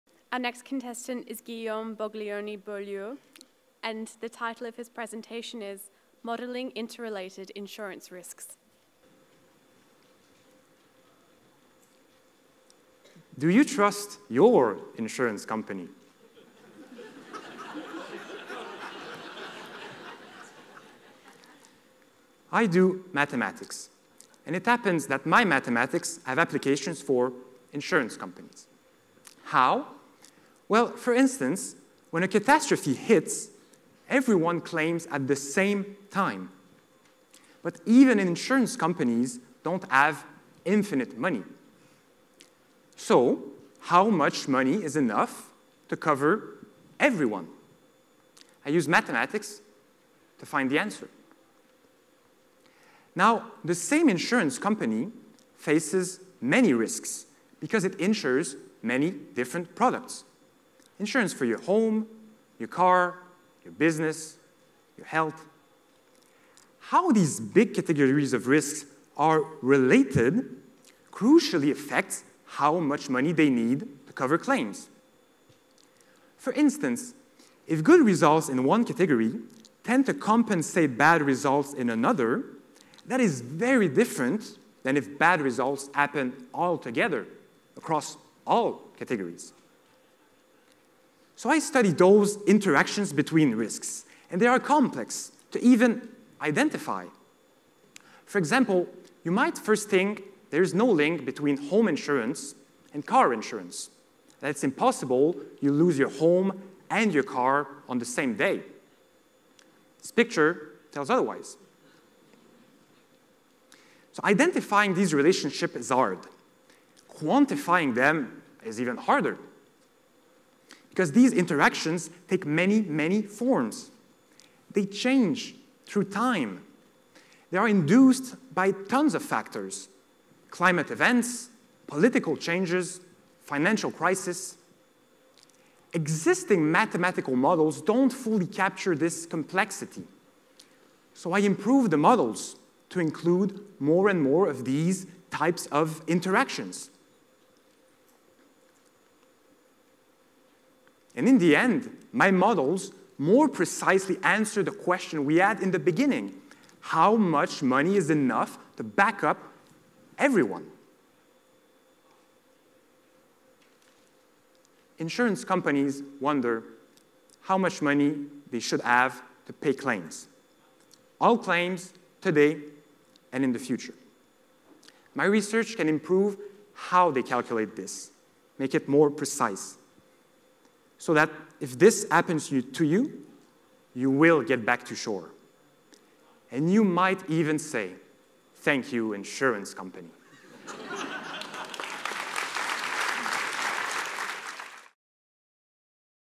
UNSW 3MT 2018 - Guillaume Boglioni Beaulieu - Modelling Interrelated Insurance Risks
UNSW Business Student Guillaume Boglioni Beaulieu gives his speech on "Modelling Interrelated Insurance Risks" for the 2018 Three Minute Thesis Final.